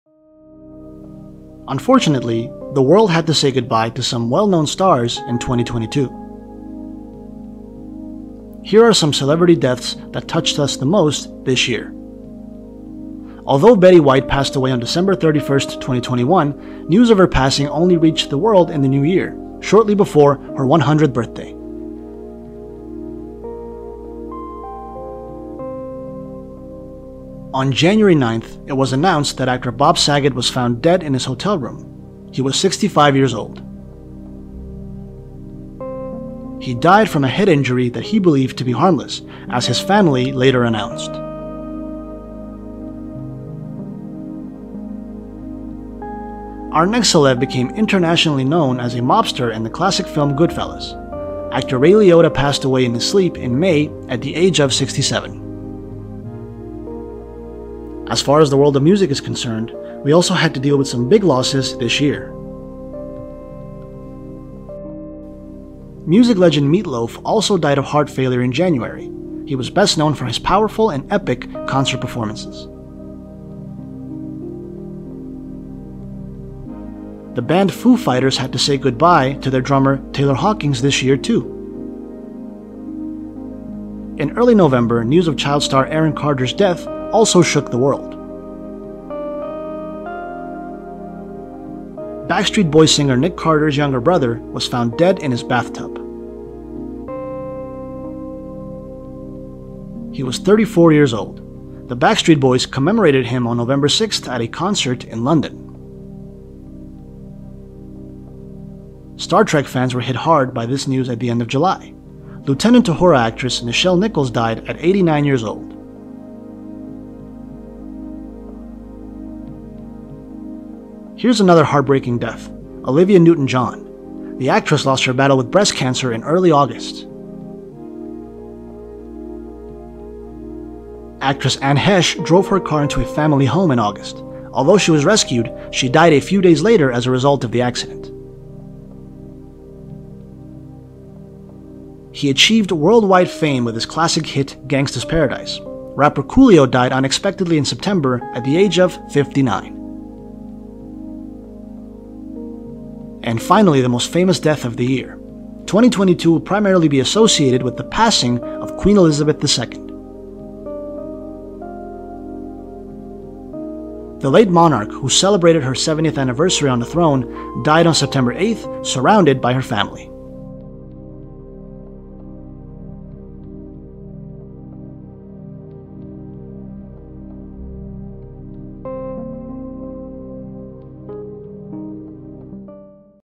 In Memoriam: The Stars We Lost In 2022 | Celebrity Hot Goss
Unfortunately, the world had to say goodbye to some well-known stars in 2022.
Here are some celebrity deaths that touched us the most this year.